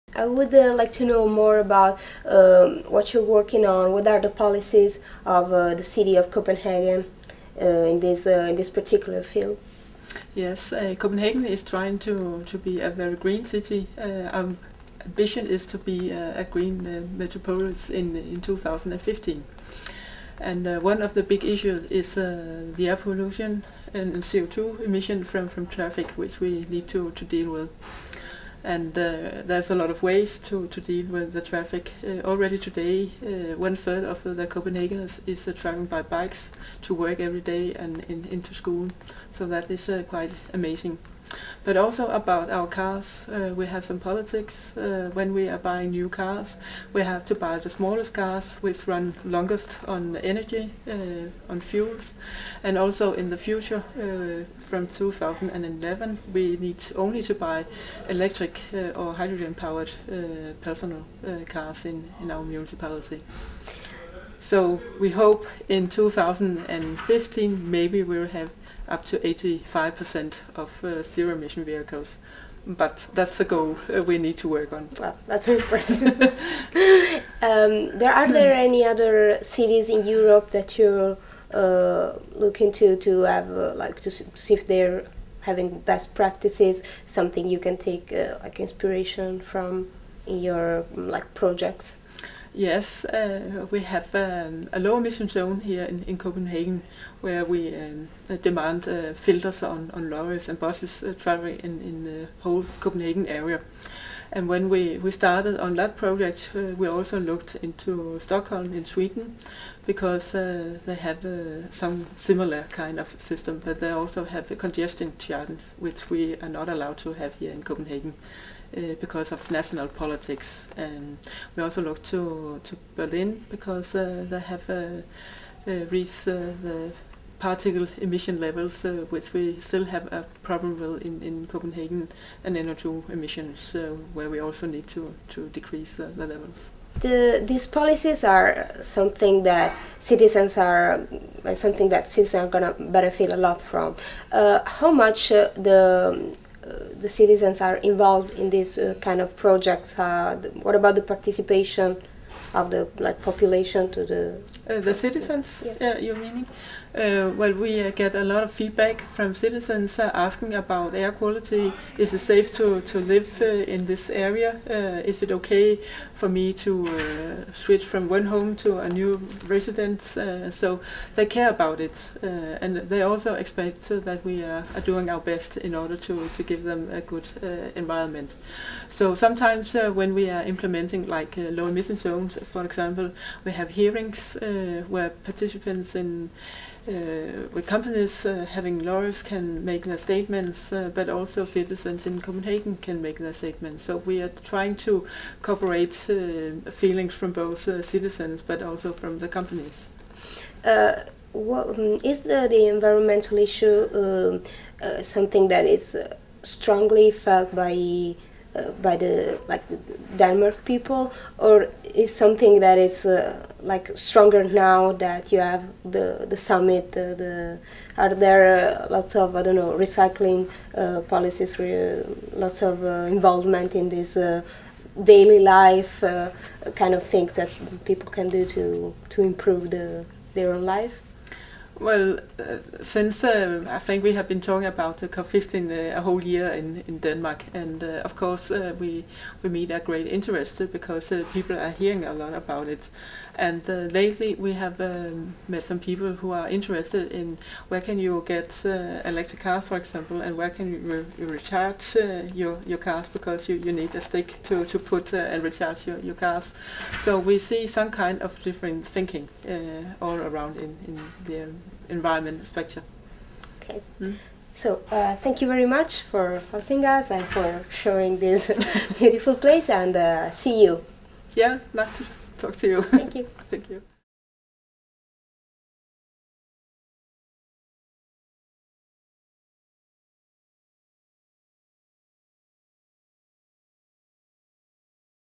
Video intervista a Birte Thomsen
Birte Thomsen, dell'Assessorato alle Politiche Ambientali di Copenhagen, intervistata da Antonella Napolitano.